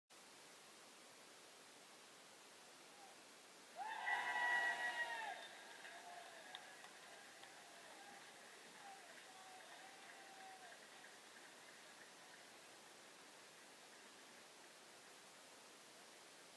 Eccellenza: Fossano-Albese 2-1, il gol di Armando che sblocca il risultato
Al quarto d'ora della ripresa Armando si inserisce bene e spedisce in gol un bel cross dalla destra di Tavella